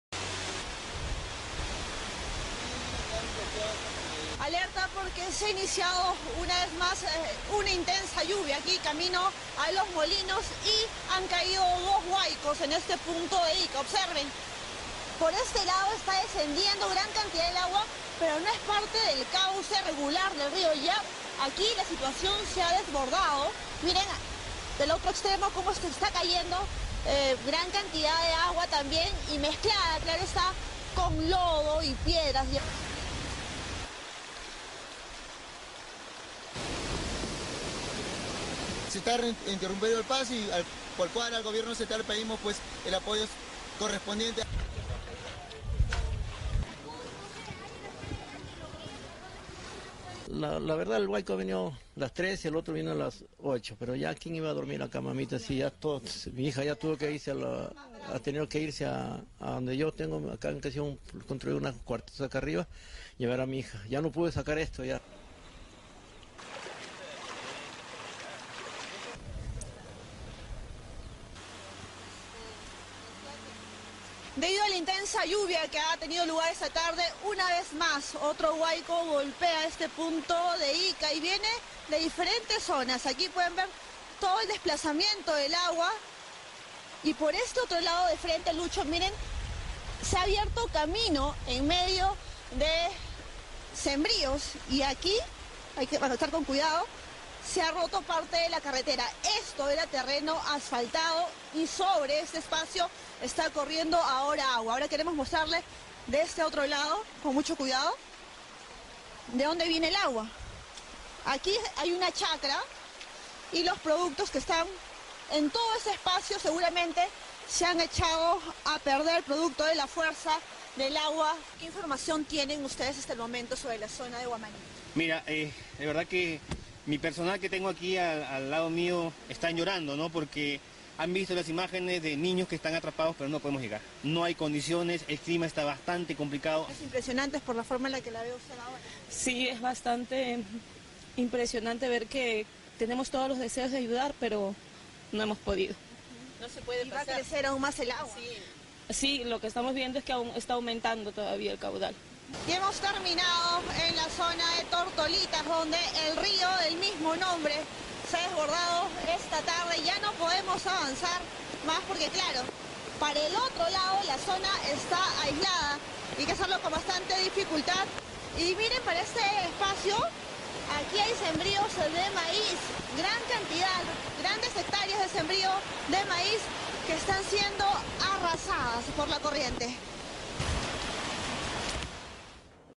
Ica: nuevo huaico cae en San José de los Molinos provocando graves daños
1

Una intensa lluvia provoc&oacute; la ca&iacute;da huaicos en la zona de San Jos&eacute; de los Molinos, ubicado en el departamento de Ica. Gran cantidad de agua, lodo y piedras es lo que pudieron captar las c&aacute;maras de 24 Horas. Esto provoc&oacute; el desborde de un r&iacute;o de la zona. Por su parte, un poblador menciona: que &ldquo;se est&aacute; interrumpiendo el paso y pide al gobierno central el apoyo correspondiente&rdquo;.

&ldquo;La verdad el huaico ha venido a las tres y el otro a las ocho, pero ya qui&eacute;n iba a dormir ac&aacute; mamita (&hellip;) mi hija se tuvo que ir a unos cuartos que tengo arriba (&hellip;) ya no pude sacar esto&rdquo;, narr&oacute; un vecino.&nbsp; Y tras las intensas precipitaciones durante la ma&ntilde;ana, un nuevo aluvi&oacute;n que cay&oacute; en horas de la tarde provoc&oacute; la destrucci&oacute;n de una carretera asfaltada, adem&aacute;s de zonas de cultivos.

Huaman&iacute;, es otra de las localidades que se han visto da&ntilde;adas por la furia de la naturaleza.&nbsp; Un equipo de personas que fueron enviadas para apoyar en la emergencia, contaron lo que pudieron presenciar. &ldquo;La verdad que mi personal que tengo aqu&iacute;, al lado m&iacute;o est&aacute;n llorando porque han visto las im&aacute;genes de ni&ntilde;os que est&aacute;n atrapados, pero no podemos llegar. No hay condiciones, el clima est&aacute; bastante complicado&rdquo; mencion&oacute; un rescatista,

Adem&aacute;s&nbsp; varios de ellos se vieron afectados emocionalmente porque a pesar de tener todos los deseos de ayudar, no pudieron concretar esa acci&oacute;n. En la zona de Tortolitas, el r&iacute;o del mismo nombre se desbord&oacute; la tarde del mi&eacute;rcoles y donde el torrente arras&oacute; con las cosechas de ma&iacute;z.